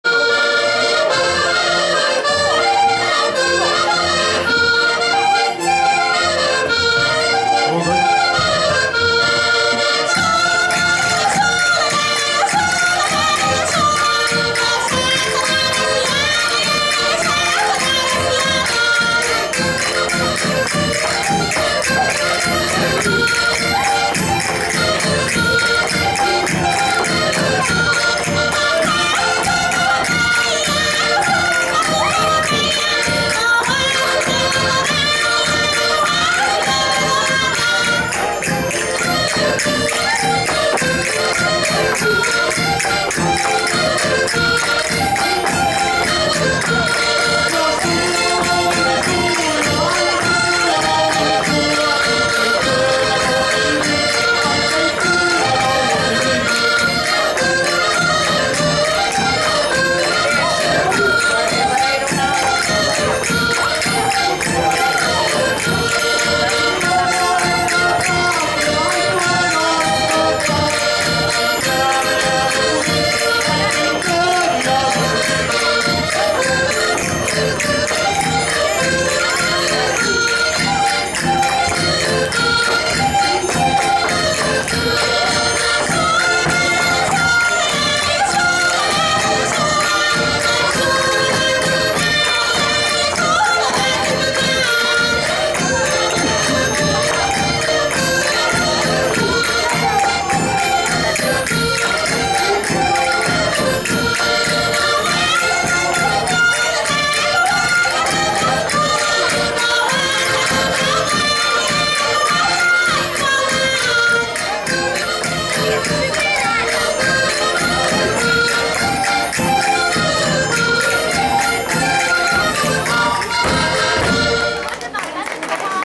Rancho Folclorico Centro Cultural de Argenteuil - Chula (28-02-2016)
"Festival de Inverno" organizado pelo Rancho Folclorico Estrelas de Portugal de Cergy-Pontoise em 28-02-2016